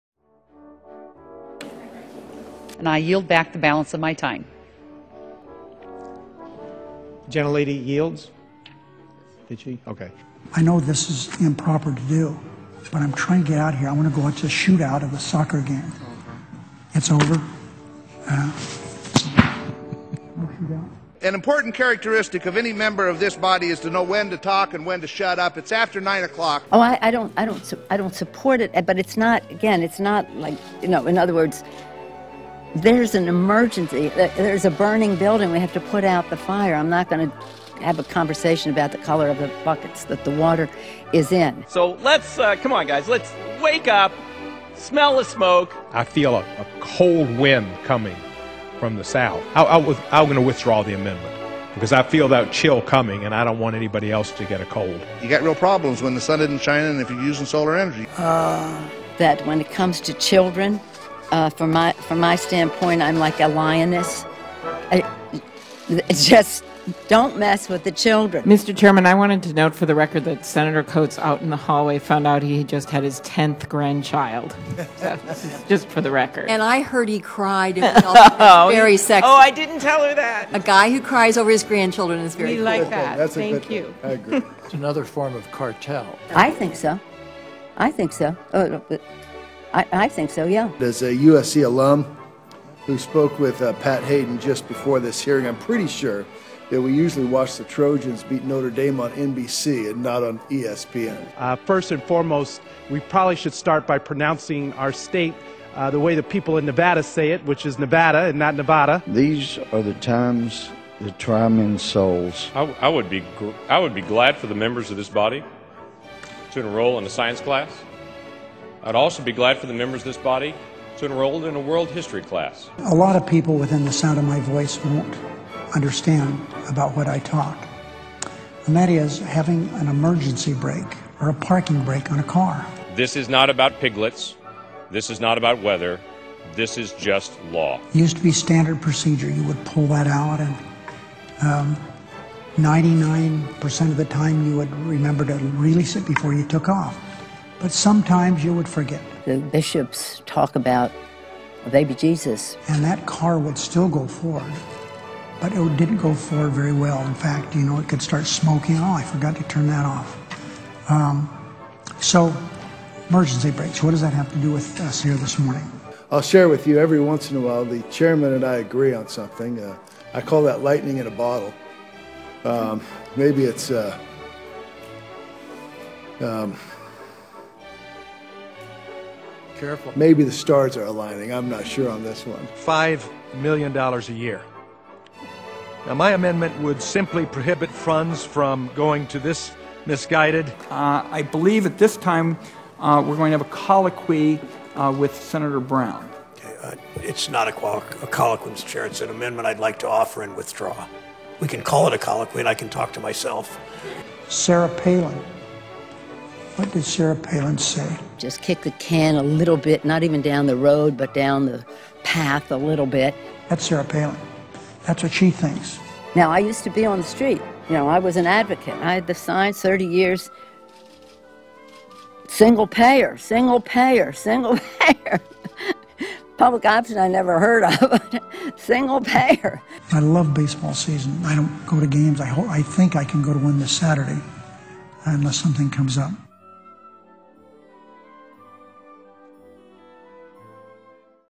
Congressional Hits and Misses: Week of July 7
As members discuss Jesus and car brakes, Rep. Mike Simpson says members need to know when to "shut up."